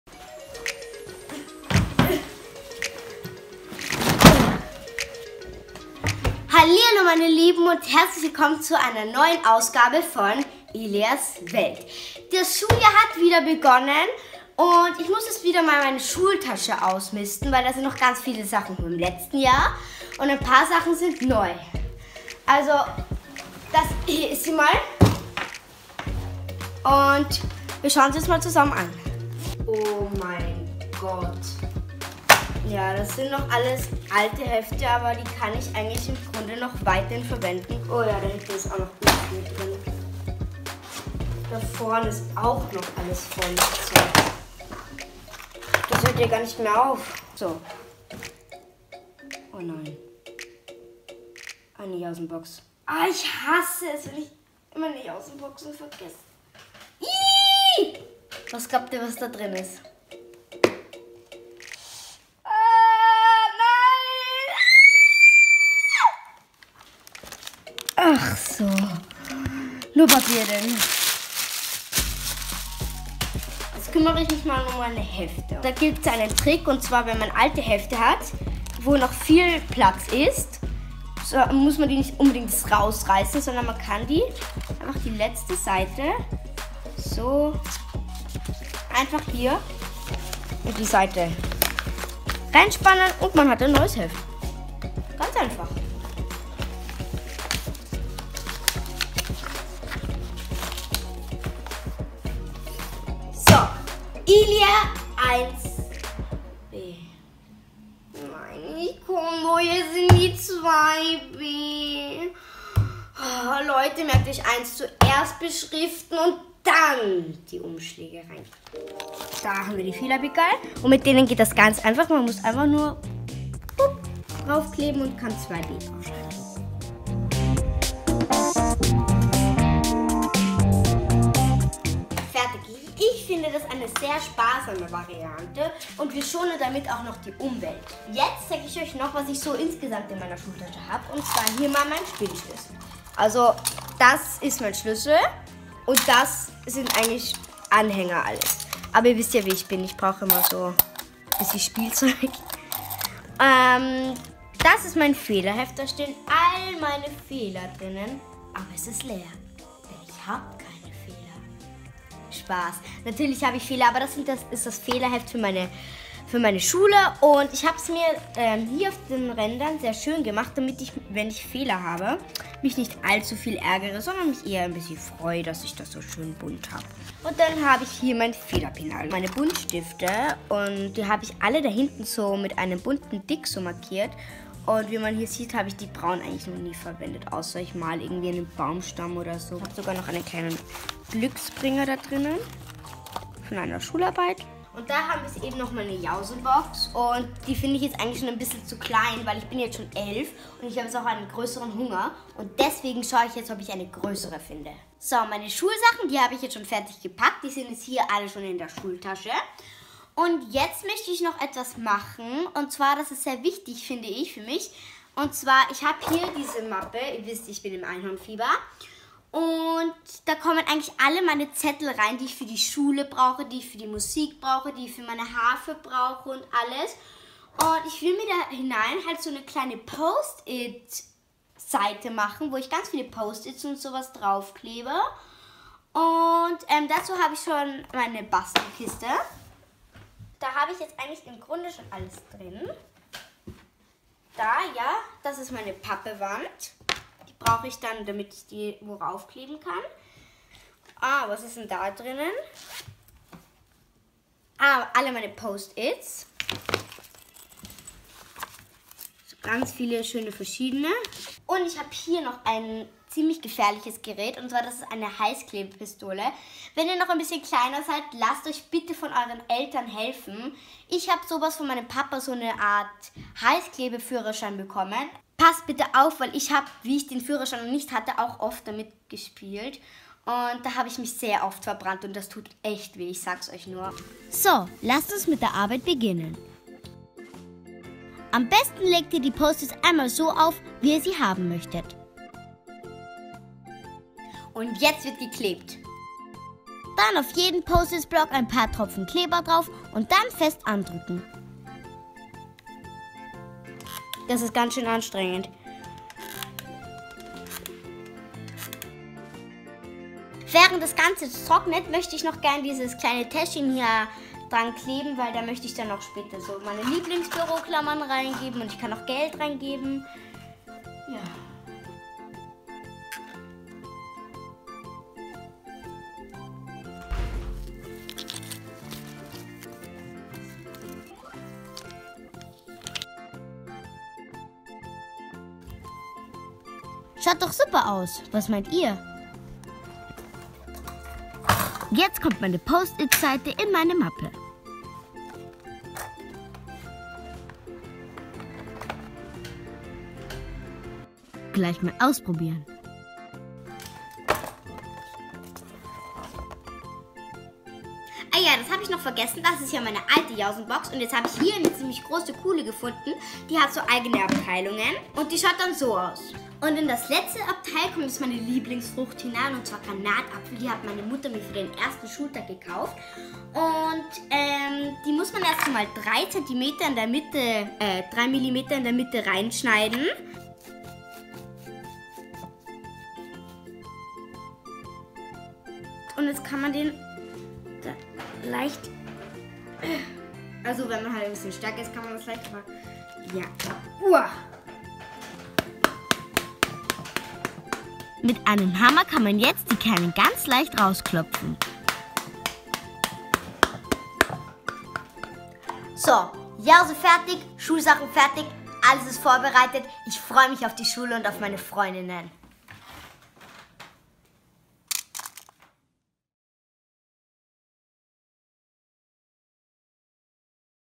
ILIAS WELT HISTORY - Vorbereitung für den ersten Schultag
So habe ich mich noch vor ein paar Jahren auf meinen ersten Schultag vorbereitet. Bald ist es bei uns wieder so weit... :(